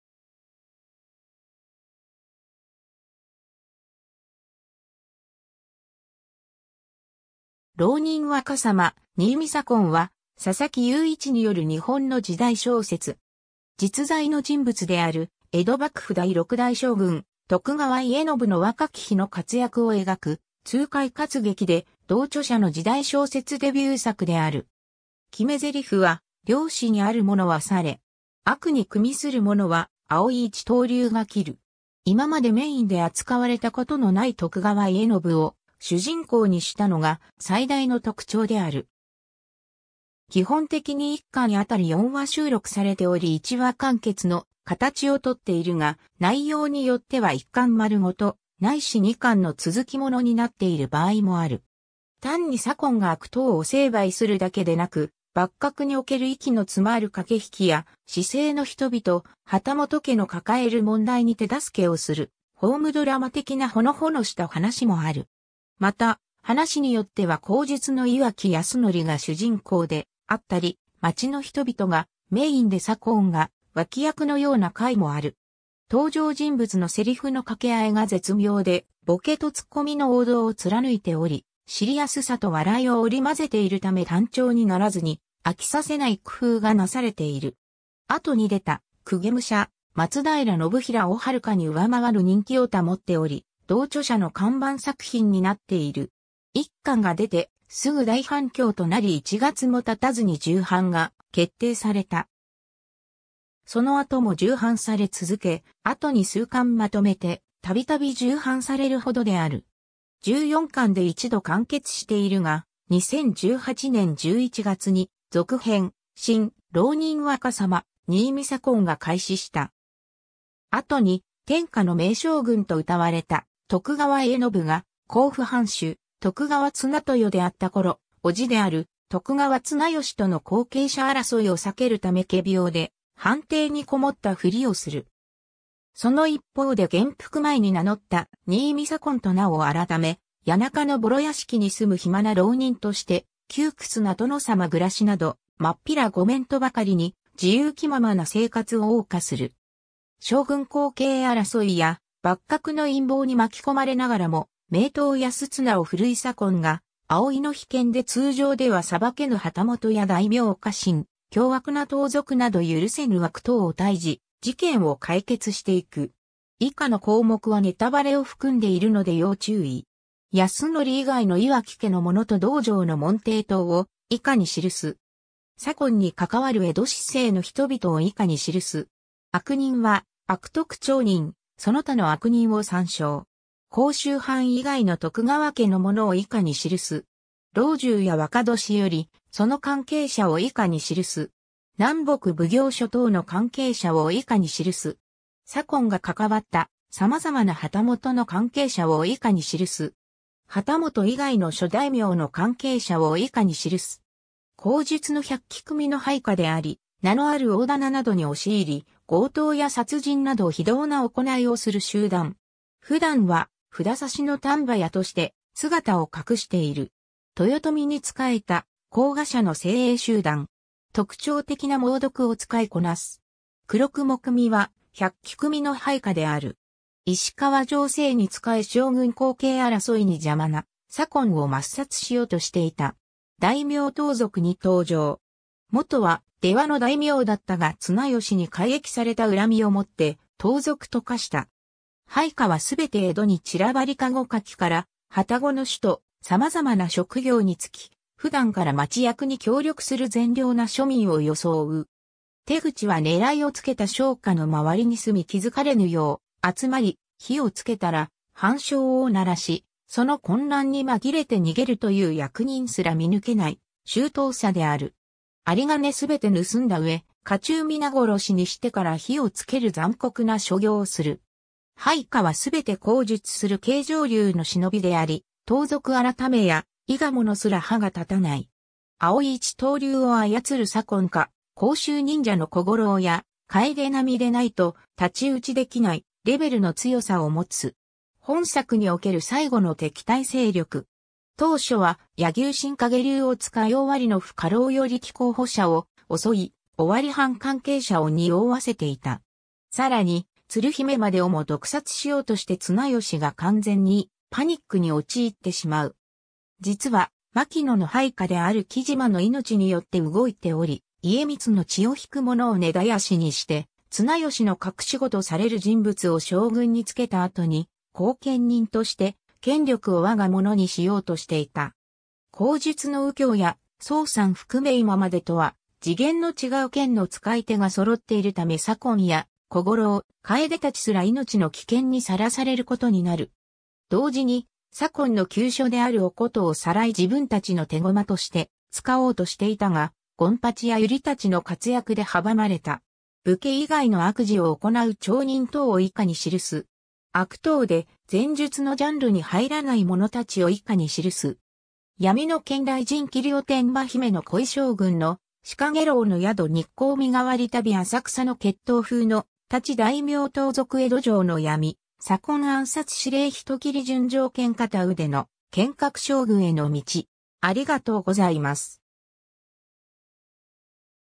浪人若さま新見左近
『浪人若さま新見左近』（ろうにんわかさま にいみさこん）は、佐々木裕一による日本の時代小説。
 実在の人物である江戸幕府第6代将軍徳川家宣の若き日の活躍を描く痛快活劇で、同著者の時代小説デビュー作である。
決め台詞は「良心ある者は去れ。悪に与する者は、葵一刀流が斬る！」 今までメインで扱われた事の無い徳川家宣を主人公にしたのが最大の特徴である。
基本的に1巻あたり4話収録されており1話完結の形を取っているが、内容によっては1巻丸ごと、ないし2巻の続き物になっている場合もある。
単に左近が悪党を成敗するだけでなく幕閣における息の詰まる駆け引きや市井の人々、旗本家の抱える問題に手助けをするホームドラマ的なほのぼのした話もある。
また、話によっては後述の岩城泰徳が主人公であったり、街の人々がメインで左近が脇役のような回もある。
登場人物のセリフの掛け合いが絶妙で「ボケと突っ込み」の王道を貫いており、シリアスさと笑いを織り交ぜているため単調にならずに飽きさせない工夫がなされている。
後に出た『公家武者松平信平』を遥かに上回る人気を保っており、同著者の看板作品になっている。
1巻が出てすぐ大反響となり一月も経たずに重版が決定された。
その後も重版され続け、後に数巻まとめて度々重版されるほどである。
14巻で一度完結しているが、2018年11月に続編「新・浪人若さま新見左近」が開始した。
 後に天下の名将軍と謳われた徳川家宣が甲府藩主・徳川綱豊であったころ、叔父である徳川綱吉との後継者争いを避けるため仮病で藩邸にこもったふりをする。
その一方で元服前に名乗った新見左近と名を改め、谷中のぼろ屋敷に住む暇な浪人として窮屈な殿さま暮らしなどまっぴら御免とばかりに、自由気ままな生活を謳歌する。
将軍後継争いや幕閣の陰謀に巻き込まれながらも名刀安綱を振るい左近が、葵の秘剣で通常では裁けぬ旗本や大名家臣、凶悪な盗賊など許せぬ悪党を退治・事件を解決していく。
 以下の項目はネタバレを含んでいるので要注意。
 泰徳以外の岩城家の者と道場の門弟等を以下に記す。
 左近に関わる江戸市井の人々を以下に記す。
悪人は、悪徳町人、その他の悪人を参照。
 甲州藩以外の徳川家の者を以下に記す。
 老中や若年寄、その関係者を以下に記す。
 南北奉行所等の関係者を以下に記す。
 左近が関わった様々な旗本の関係者を以下に記す。
 旗本以外の諸大名の関係者を以下に記す。
 後述の百鬼組の配下であり、名のある大店などに押し入り強盗や殺人など非道な行いをする集団。
普段は札差の丹波屋として姿を隠している。
 豊臣に仕えた甲賀者の精鋭集団。
特徴的な猛毒を使いこなす。
黒蜘蛛組は百鬼組の配下である。
石川乗政に仕え将軍後継争いに邪魔な左近を抹殺しようとしていた。
 「大名盗賊」に登場。
元は出羽の大名だったが綱吉に改易された恨みを持って盗賊と化した。
配下は全て江戸に散らばり駕籠かきから旅籠の主と様々な職業につき普段から町役に協力する善良な庶民を装う。
手口は狙いをつけた商家の周りに住み気づかれぬよう集まり、火を付けたら半鐘を鳴らし、その混乱に紛れて逃げるという役人すら見抜けない周到さである。
有り金全て盗んだ上、家中皆殺しにしてから火をつける残酷な所業をする。
配下は全て後述する経上流の忍びであり盗賊改や伊賀者すら歯が立たない。
葵一刀流を操る左近か甲州忍者の小五郎やかえで並みでないと太刀打ちできないレベルの強さを持つ。
 本作における最後の敵対勢力。
当初は柳生新陰流を使い尾張の付家老与力候補者を襲い、尾張藩関係者をにおわせていた。
更に鶴姫までをも毒殺しようとして綱吉が完全にパニックに陥ってしまう。
実は牧野の配下である木島の命によって動いており、家光の血を引く者を根絶やしにして綱吉の隠し子とされる人物を将軍に就けた後に、後見人として権力を我が物にしようとしていた。
後述の右京や奏山含め今までとは次元の違う剣の遣い手が揃っているため左近や小五郎、かえで達すら命の危険にさらされることになる。
同時に左近の急所であるお琴を攫い自分たちの手駒として使おうとしていたが権八や百合達の活躍で阻まれた。
 武家以外の悪事を行う町人等を以下に記す。
 悪党で前述のジャンルに入らない者達を以下に記す。
 闇の剣（2010年） 雷神斬り（2010年） おてんば姫の恋（2011年） 将軍の死（2012年） 陽炎の宿（2012年） 日光身代わり旅 (2014年) 浅草...